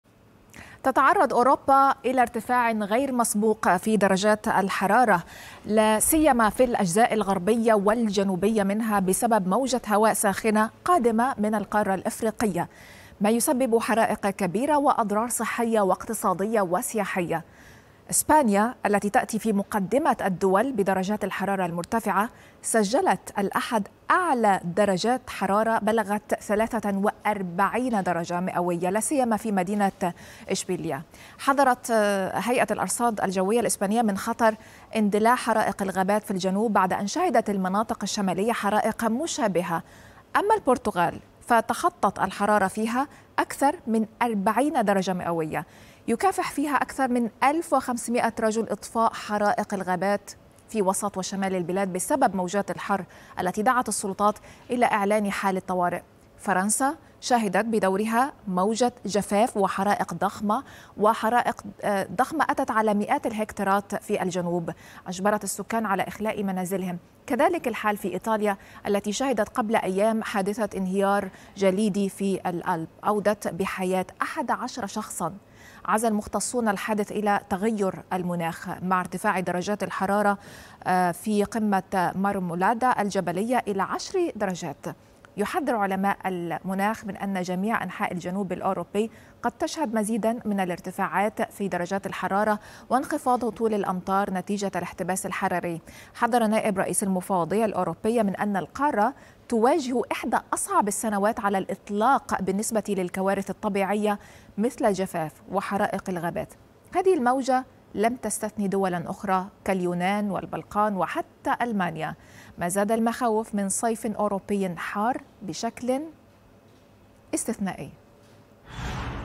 صيف لاهب في أوروبا.. جفاف وحرائق ودرجات أربعينية
جفاف وحرائق وانهيارات ثلجية.. صيف لاهب في أوروبا.. الدرجات أربعينية وتحذيرات من ارتفاعات قادمة